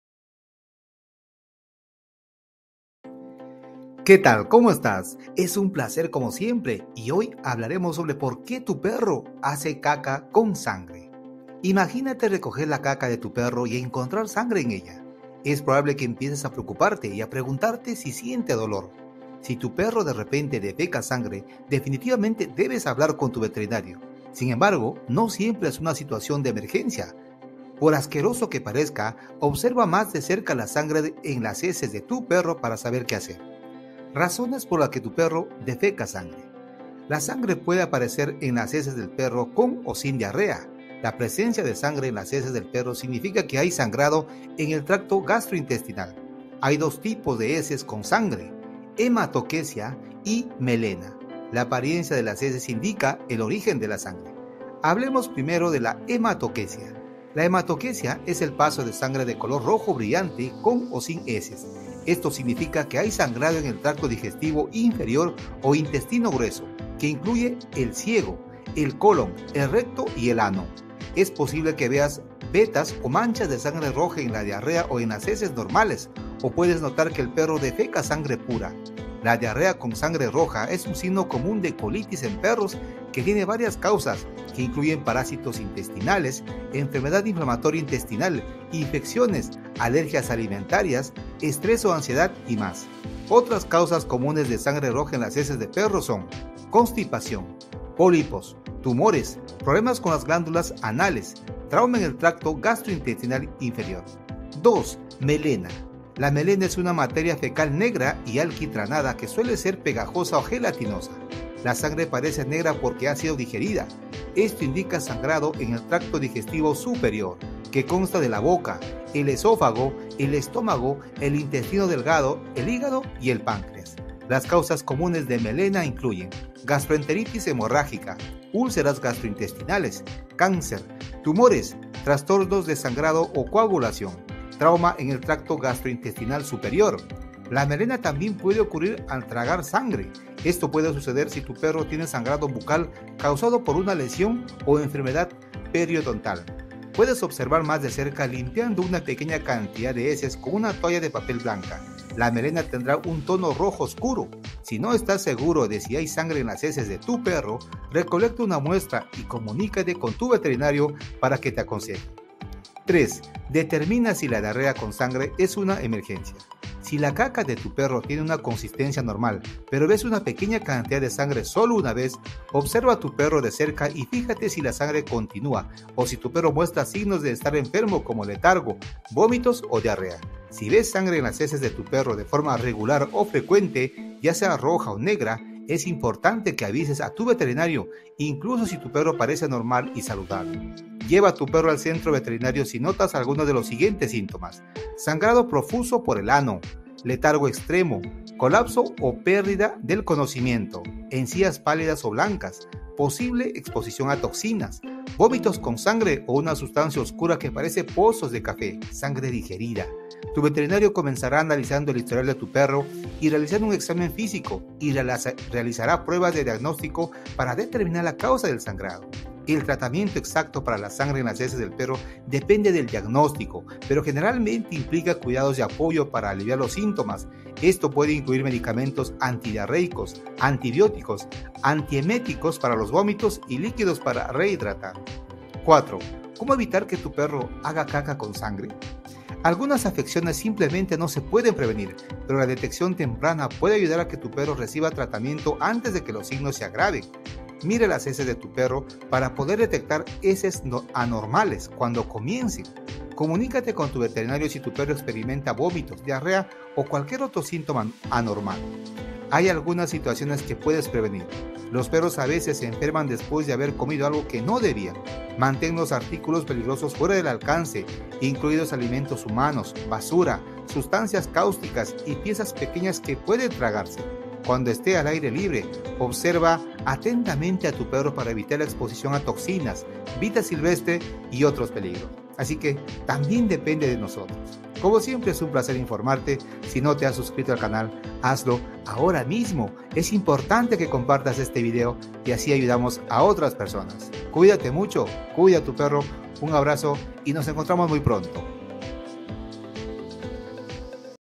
¿POR QUÉ mi PERRO hace CACA con SANGRE?🩸💩
📌Imagínate recoger la caca de tu perro y encontrar sangre en ella. Es probable que empieces a preocuparte y a preguntarte si siente dolor. Si tu perro de repente defeca sangre, definitivamente debes hablar con tu veterinario.

Sin embargo, no siempre es una situación de emergencia. Por asqueroso que parezca, observa más de cerca la sangre en las heces de tu perro para saber qué hacer... MIRA EL VIDEO.